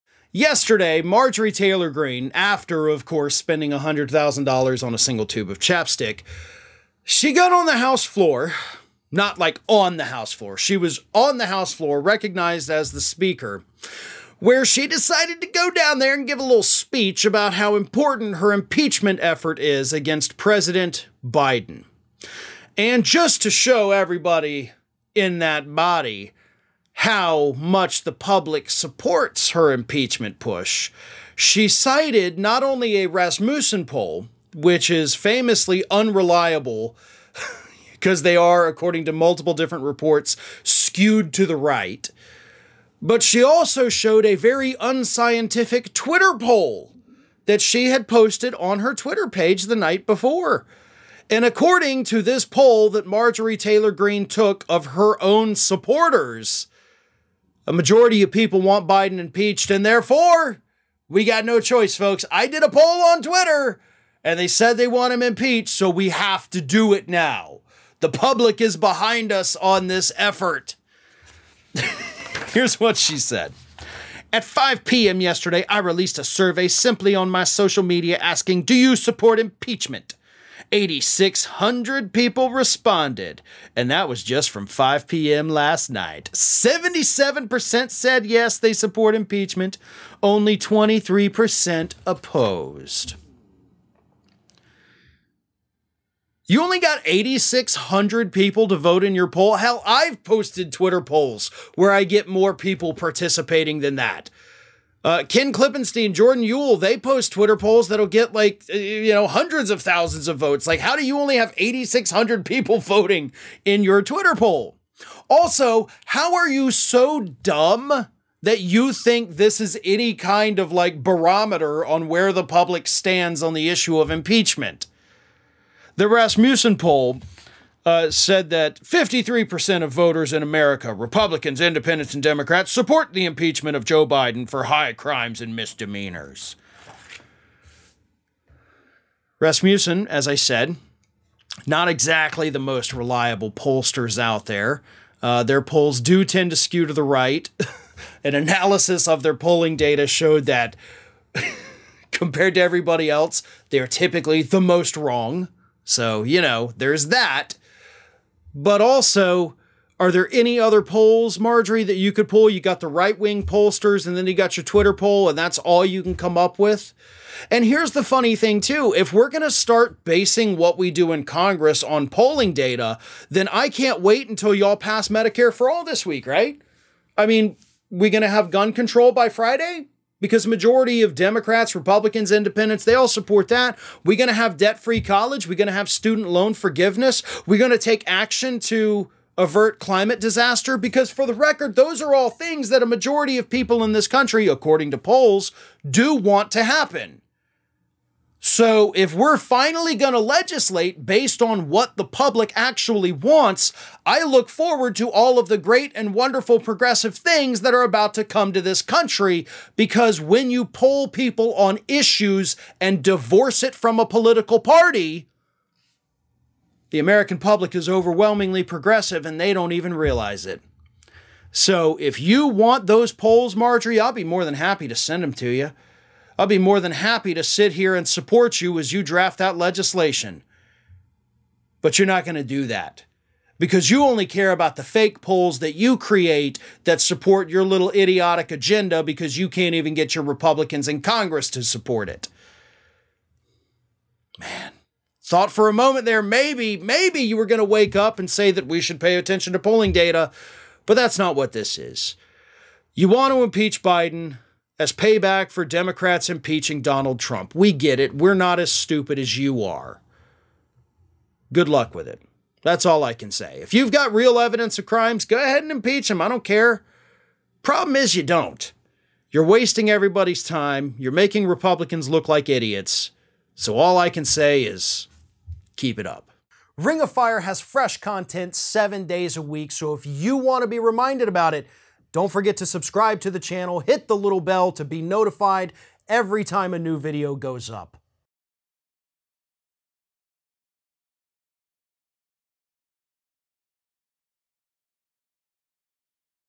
Marjorie Taylor Greene Cites Her Own Twitter Poll As Justification For Her Impeachment Crusade
Marjorie Taylor Greene is still trying to move forward with her attempts to impeach everyone from US Attorneys to President Biden, and she claimed during a floor speech on Tuesday that a completely unreliable poll that she posted to Twitter is proof that the public wants Biden impeached. She also cited a poll from Rasmussen, which is barely more reliable than her Twitter poll. The lunatics are officially running the asylum, as Ring of Fire's Farron Cousins explains.

Yesterday, Marjorie Taylor Green, after of course spending a hundred thousand dollars on a single tube of Chapstick. She got on the house floor, not like on the house floor. She was on the house floor, recognized as the speaker where she decided to go down there and give a little speech about how important her impeachment effort is against President Biden. And just to show everybody in that body how much the public supports her impeachment push. She cited not only a Rasmussen poll, which is famously unreliable cuz they are, according to multiple different reports skewed to the right. But she also showed a very unscientific Twitter poll that she had posted on her Twitter page the night before. And according to this poll that Marjorie Taylor Green took of her own supporters, a majority of people want Biden impeached and therefore we got no choice folks.

I did a poll on Twitter and they said they want him impeached, so we have to do it now. The public is behind us on this effort, here's what she said. 8,600 people responded, and that was just from 5:00 PM last night. 77% said yes, they support impeachment only 23% opposed, you only got 8,600 people to vote in your poll. Hell, I've posted Twitter polls where I get more people participating than that. Uh, Ken Klippenstein, Jordan Yule, they post Twitter polls that'll get like, you know, hundreds of thousands of votes. Like how do you only have 8,600 people voting in your Twitter poll? Also, how are you so dumb that you think this is any kind of like barometer on where the public stands on the issue of impeachment?

The Rasmussen poll, uh, said that 53% of voters in America, Republicans, independents, and Democrats support the impeachment of Joe Biden for higher crimes. And misdemeanors, Rasmussen, as I said, not exactly the most reliable pollsters out there. Uh, their polls do tend to skew to the right. An analysis of their polling data showed that compared to everybody else, they're typically the most wrong. So, you know, there's that. But also are there any other polls, Marjorie, that you could pull? You got the right wing pollsters and then you got your Twitter poll and that's all you can come up with. And here's the funny thing too. If we're gonna start basing what we do in Congress on polling data, then I can't wait until y'all pass Medicare for all this week, right? I mean, we gonna have gun

Control by Friday because majority of Democrats, republicans, independents, they all support that. We gonna have debt free college, we're gonna have student loan forgiveness. We're gonna take action to avert climate disaster because for the record, those are all things that a majority of people in this country, according to polls do want to happen. So if we're finally gonna legislate based on what the public actually wants, I look forward to all of the great and wonderful progressive things that are about to come to this country. Because when you pull people on issues and divorce it from a political party, the American public is overwhelmingly progressive and they don't even realize it. So if you want those polls, Marjorie, I'll be more than happy to send them to you. I'll be more than happy to sit here and support you as you draft that legislation.